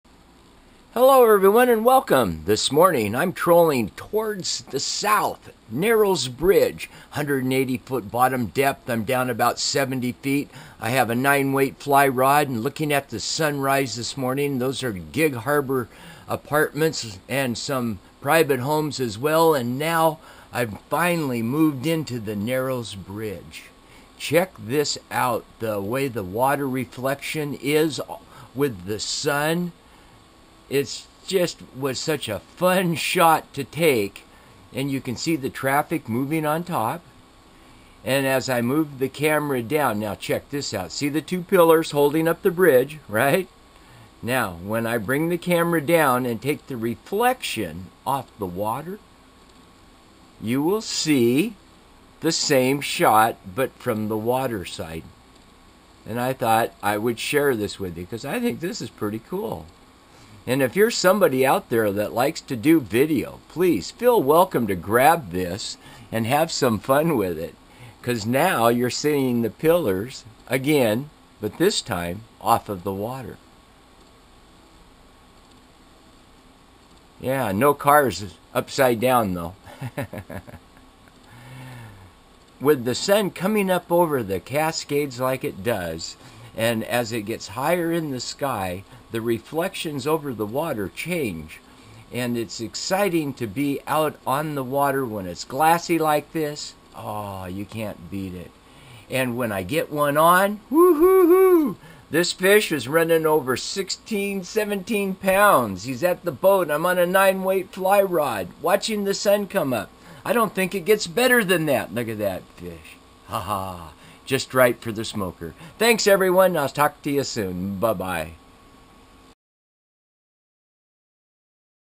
Narrows Bridge
Hello, Doc bringing you a first... The lighting is one in a million coming from the sun light as it rises up. The water also one in a million supper flat and lucked out on the timing to get the reflection shot coming off the water back up to the bridge.
I went back into my video library looking for more recordings from past years and found some more cool shots. Morning sun sises as were reeling in King salmon. "Some Kings over 60lbs". So stand by; More sun rises and salmon catching. I want to take this moment to give a big thank you to all viewers.
All the very best to you and all of yours "FOREVER". Merry Christmas and a Happy Holiday to all.
Your friend Doc Thoke "Ghost Net Guy"